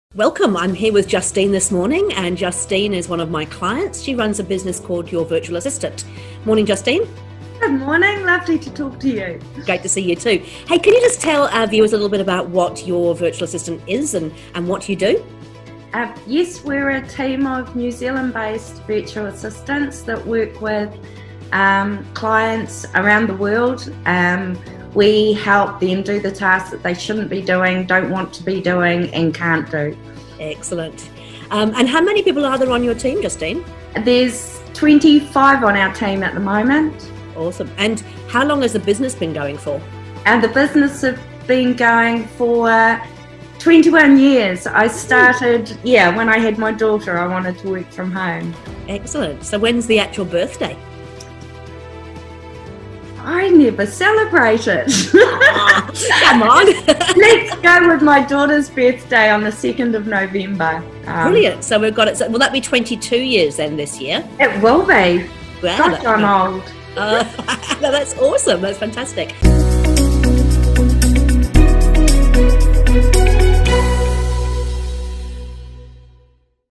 Meet Justine, a Client Story | Professional EOS Implementer | Business Traction | Debra Chantry
Do you know when your birthday is? 🎂

Of course, you do! You may enjoy big functions with balloons and cake to celebrate, others may like small gathers or even the chance to have a day off and to yourself. 🎈

Either way, birthdays are important. They give us a chance to reflect on what we have achieved and who we have become but, do you do the same for your business?

Today I ask my client Justine Parsons about her business, and how it’s grown over the years. This is the first of a series of videos featuring Justine and how she now celebrates every birthday.

It is crazy to think back to those first few years of your business growing so quick and hopefully walking on its own feet.

Maybe your business is more mature now and moving out of your first headquarter and setting up around the New Zealand or even the wider world! 🌏

Be it a wide-eyed toddler, or well into adulthood, our business still deserves a celebration.

They are our baby’s after all!

What do you to celebrate your business’ birthday?  🥳

Do you know when it is?

𝐼’𝒹 𝓁𝑜𝓋𝑒 𝓉𝑜 𝒽𝑒𝒶𝓇 𝓌𝒽𝒶𝓉 (𝒾𝒻) 𝓎𝑜𝓊 𝒹𝑜 𝓉𝑜 𝒸𝑒𝓁𝑒𝒷𝓇𝒶𝓉𝑒 𝓎𝑜𝓊𝓇 𝒷𝓊𝓈𝒾𝓃𝑒𝓈𝓈'𝓈 𝒷𝒾𝓇𝓉𝒽𝒹𝒶𝓎, 𝒾𝓃 𝓉𝒽𝑒 𝒸𝑜𝓂𝓂𝑒𝓃𝓉𝓈 🥳